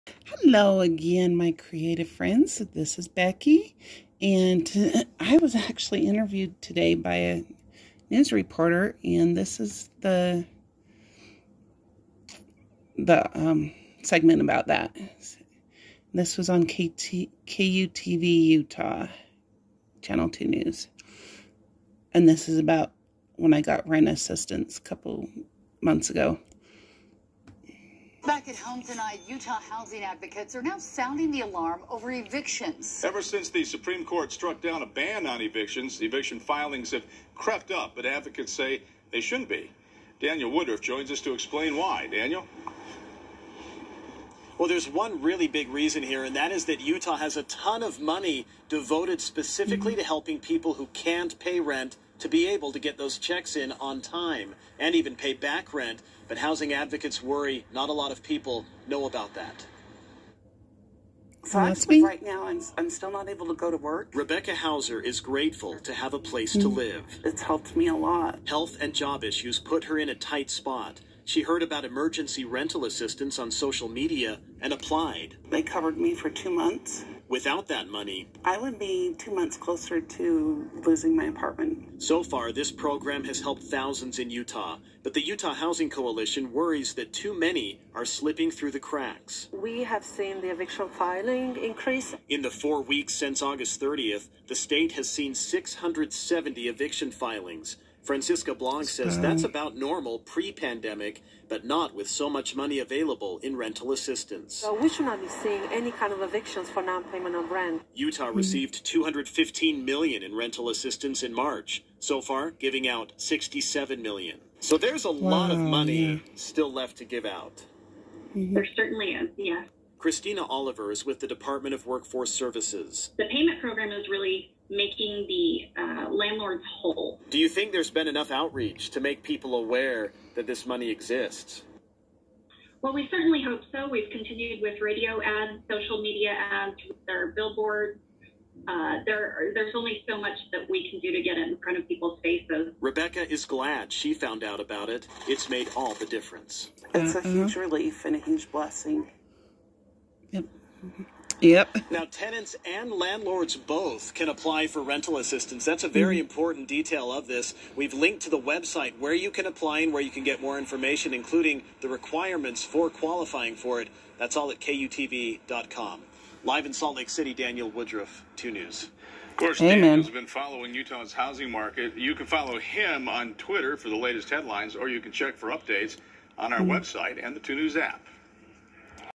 I was on the News today!
A few months ago I commented on KUTV's FB page about getting Pandemic Rent Assistance here in Utah, and was later contacted by Reporter Daniel Woodruff. I was interviewed today, and this is the segment they did.
If you are in need of rent assistance, especailly due to the Pandemic, it is out there. The website to apply here in Utah is rentrelief utah.gov. You can also contact 211 for information about agencies in your area that can help.